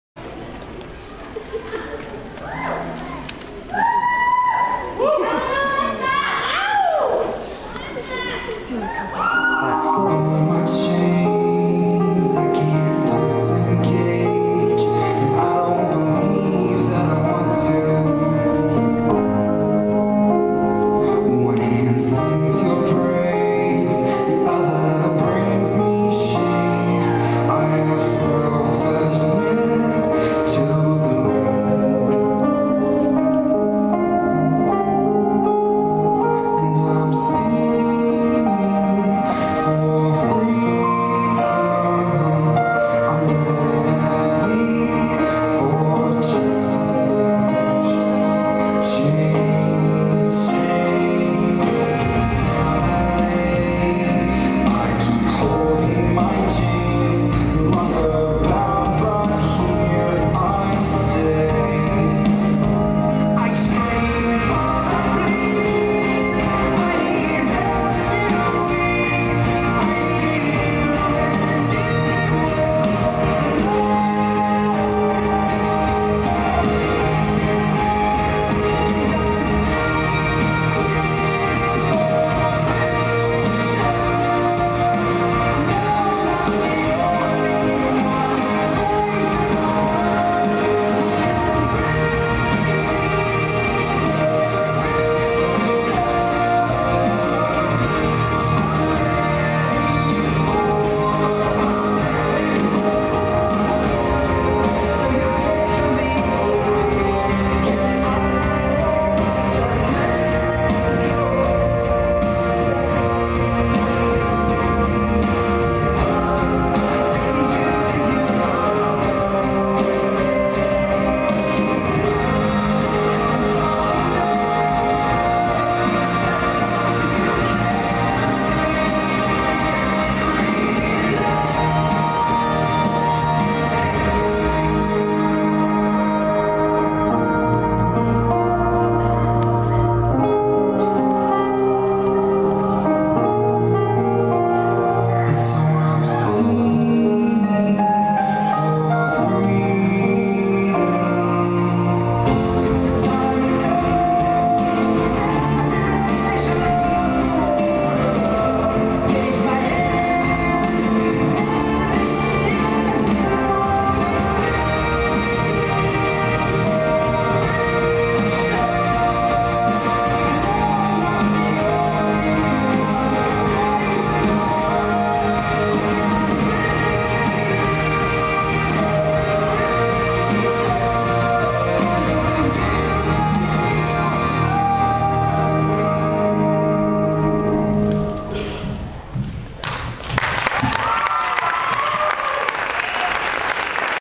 Thompson Lane, 2010 Spring concert (freedom)
Jenn Newman's Dance (freedom) is a semi lyrical dance w/ tons of passion, and emotion. (danced by TL Senior company)